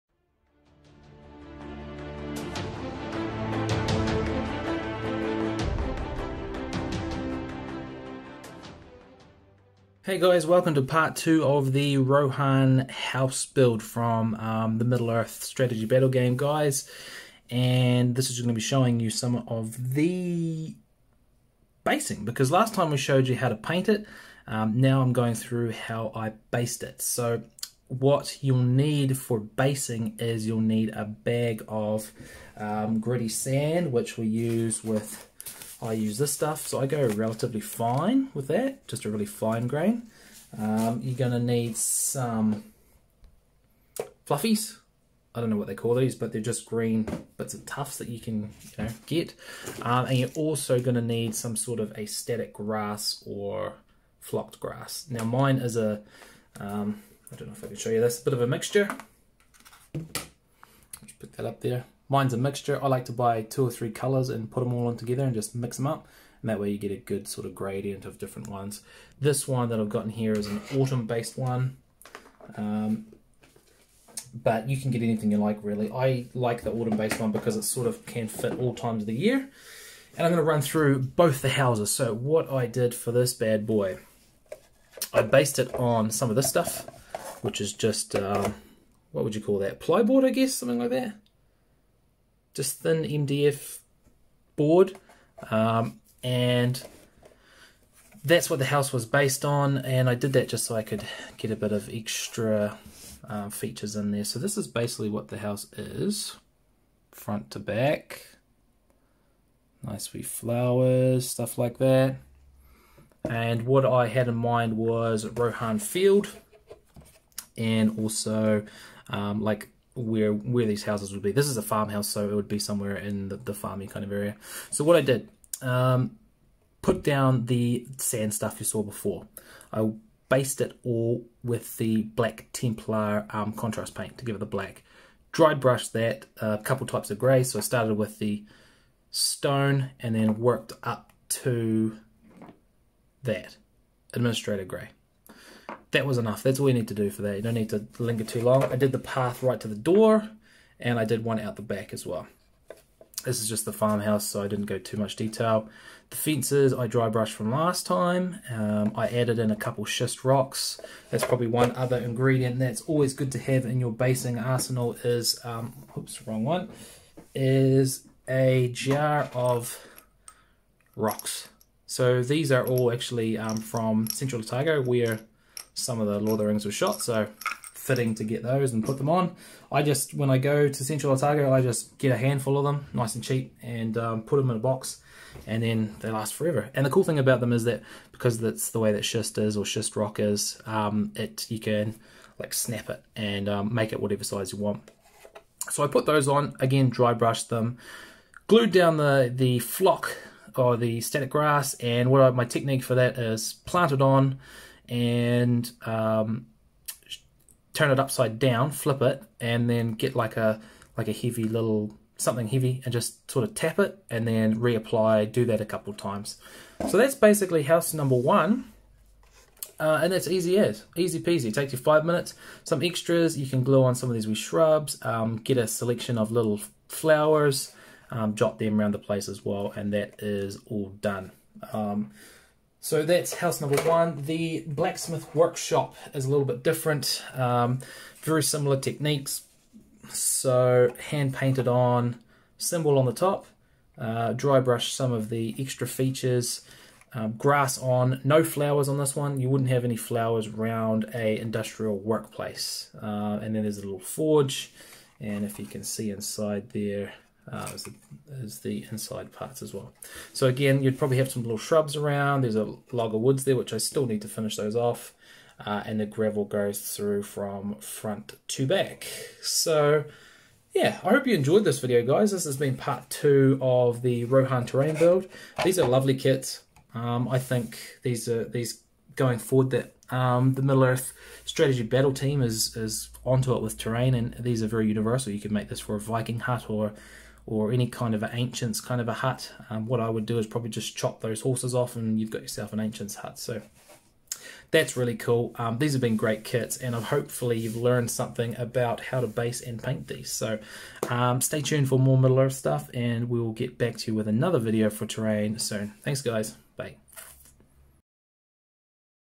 MESBG-ROHAN HOUSE PART 2
The second video showig off the Rohan House kits. This time i am going over how I finished off the basing.

Stick around for more Middle earth SBG videos and reviews.
As always Please Like and Subsrcibe.